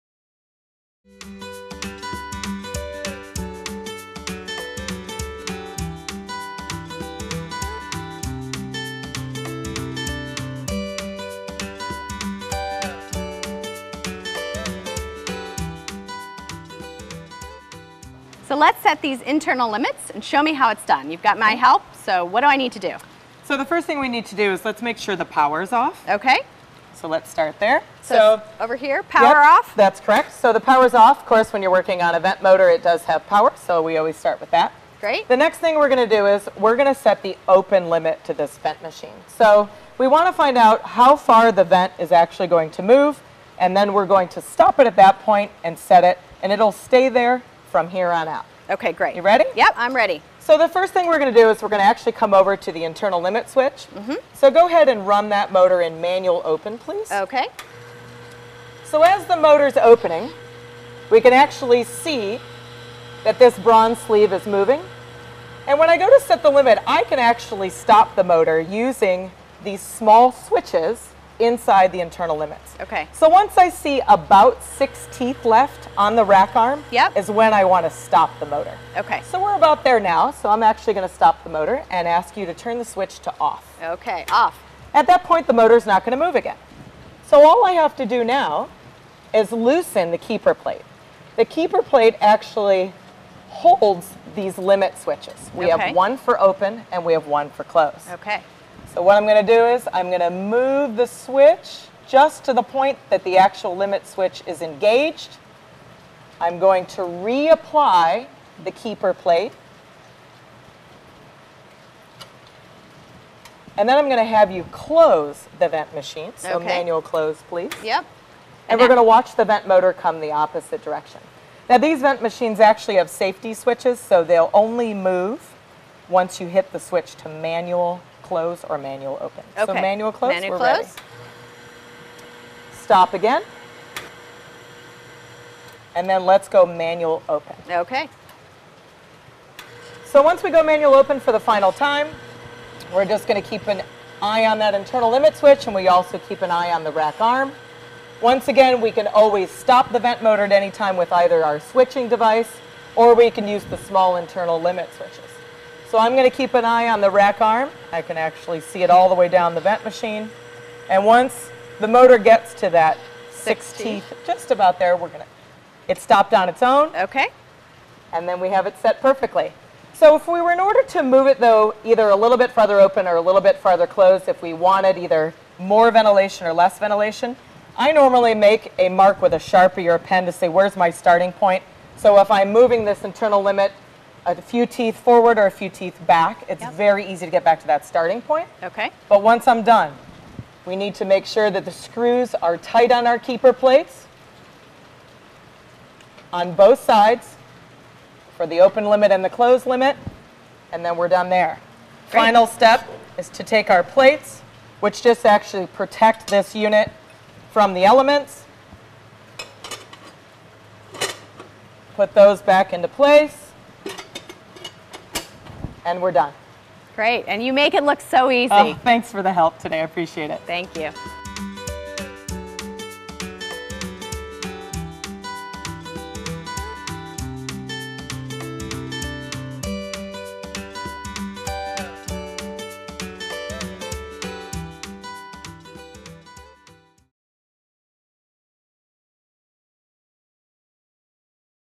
Wadsworth Controls Systems Series: Setting Internal Limits
Clara Coleman stops by Donabedian Farms in Salem, NH to speak with Amanda Debevc of Wadsworth Controls Systems. The two give a  step-by-step rundown of how to optimize your growing environment by setting the internal limits on your vent machine.